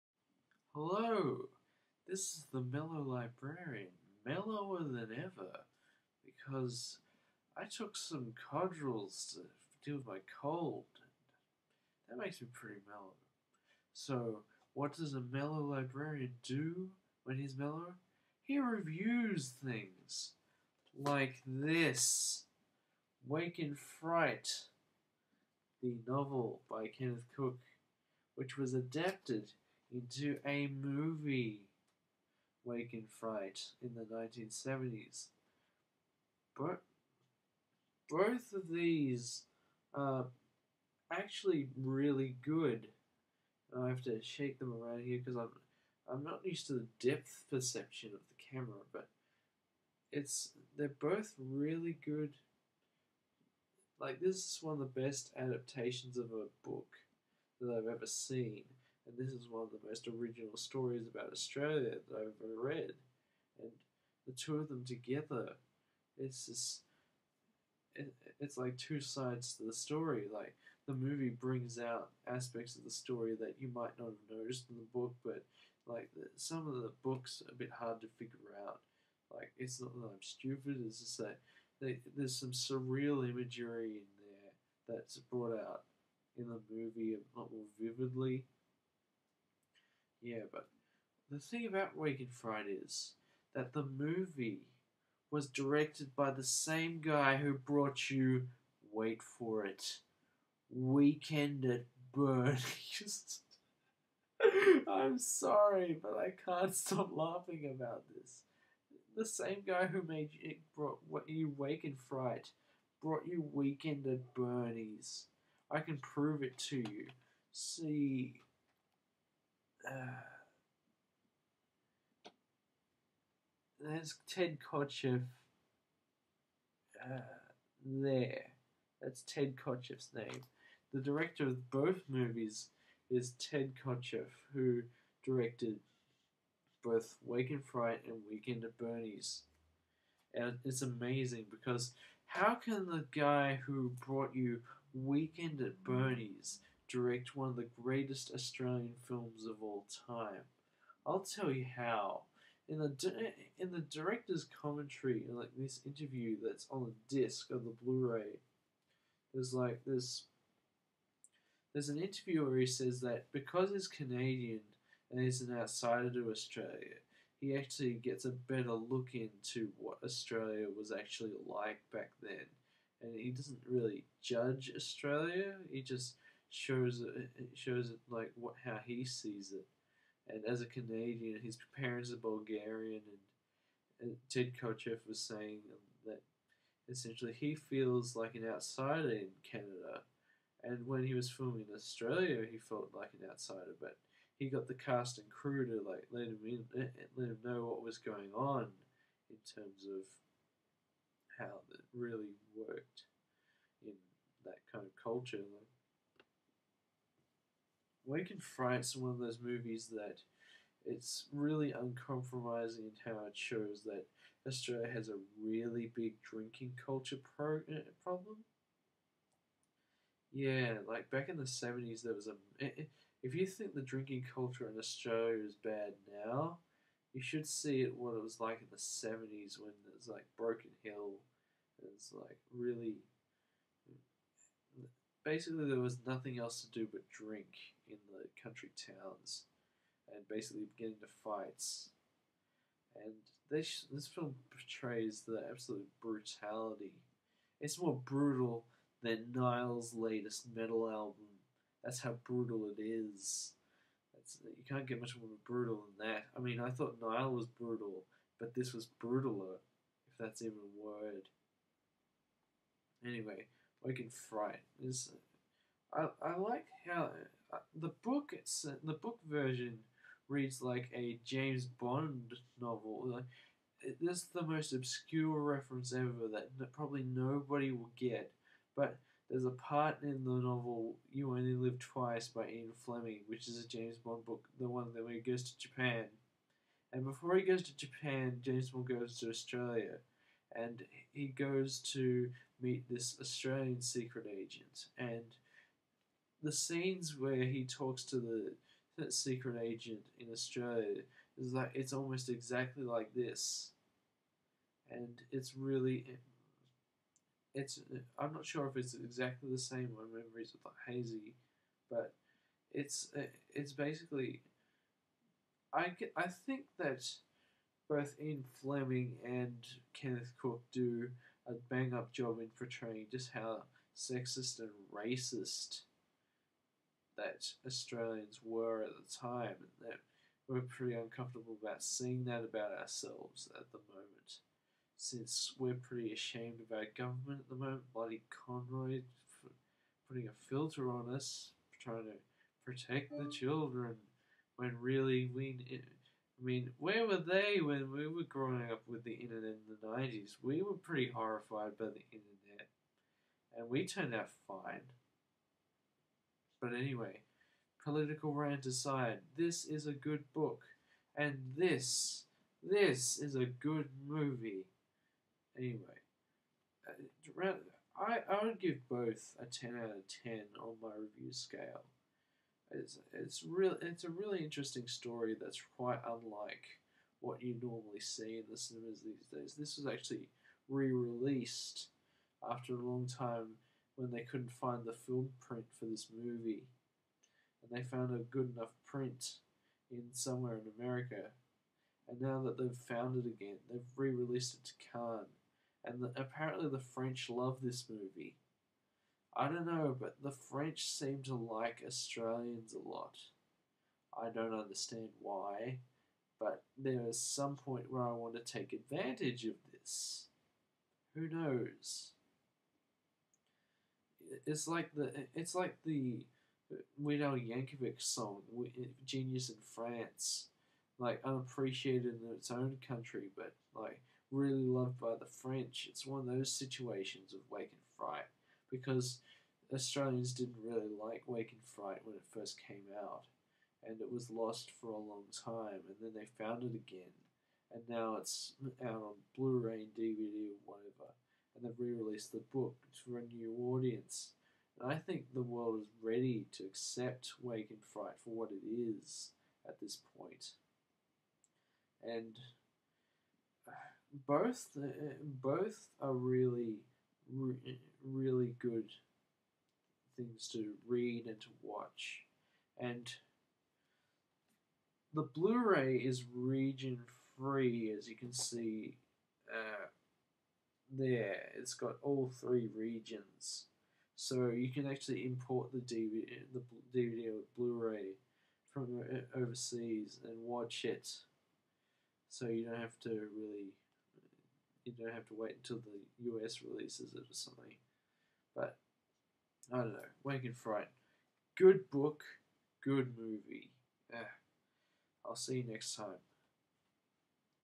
Mellow Librarian - Wake in Fright Book and Movie Review
In which I review both the movie and the book of Wake In Fright. Sorry this took so long to upload, but like the film of Wake In Fright, the footage for this review was once thought LOST but has since been rediscovered!